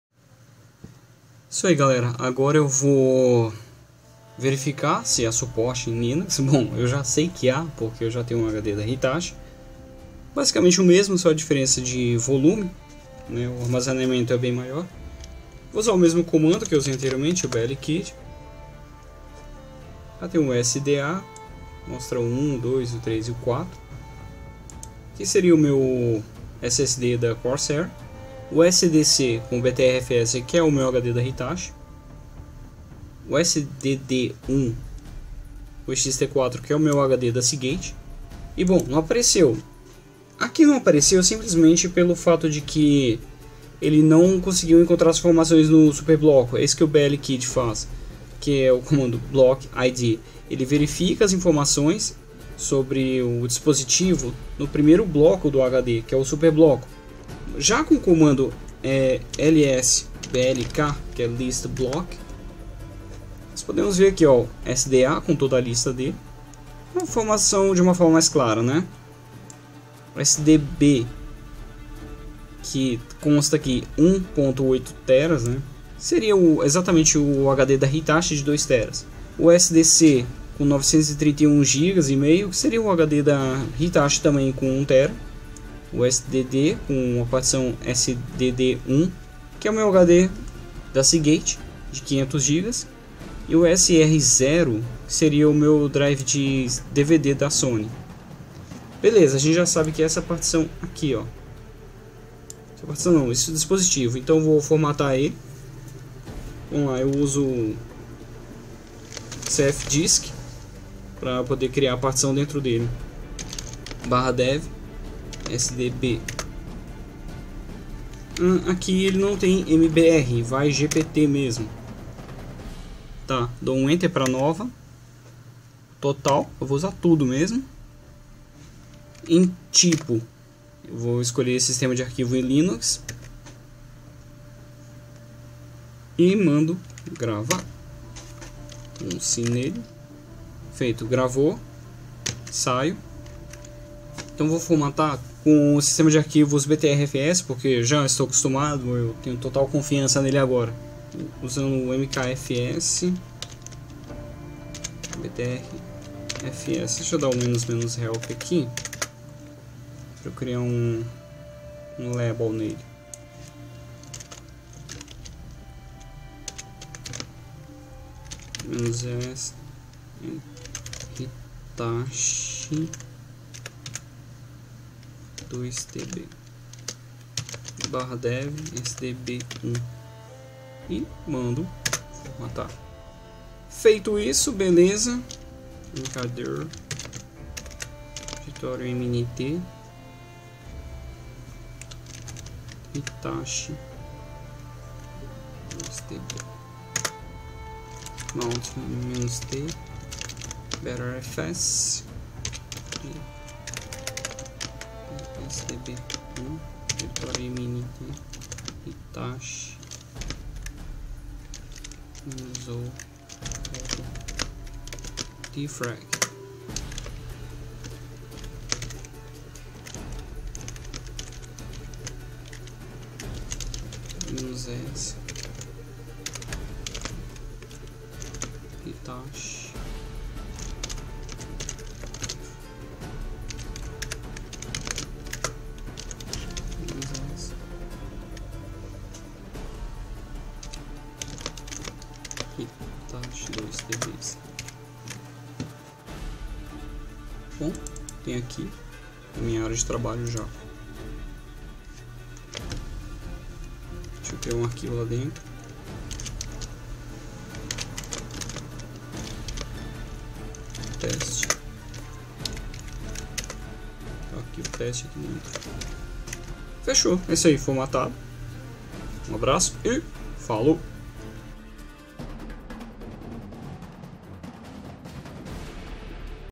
Unboxing HD Hitachi #2
Continuando o unboxing do HD Hitachi. agora populando o HD com um file system e testando.